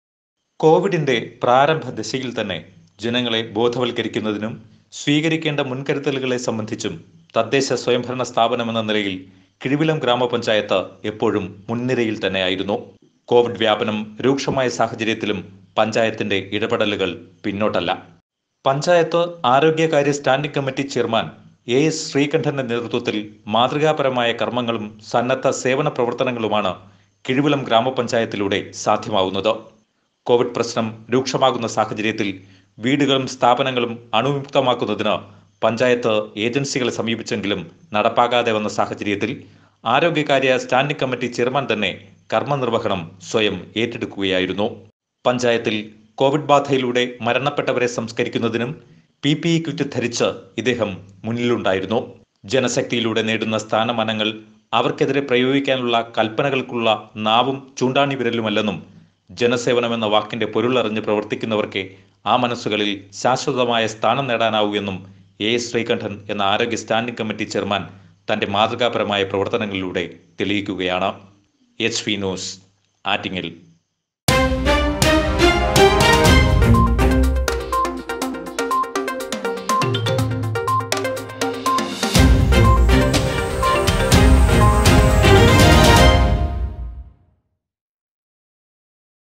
കോവിഡ് പ്രതിരോധ പ്രവർത്തനങ്ങളിൽ മാതൃകാപരമായ പ്രവർത്തനങ്ങളുമായി എ എസ് ശ്രീകണ്ഠൻ
കോവിഡ് പ്രതിരോധ പ്രവർത്തനങ്ങളിൽ മാതൃകാപരമായ പ്രവർത്തനങ്ങളുമായി കിഴുവിലം ഗ്രാമപഞ്ചായത്ത് ആരോഗ്യസ്റ്റാന്ഡിങ് കമ്മിറ്റി ചെയർമാൻ എ എസ് ശ്രീകണ്ഠൻ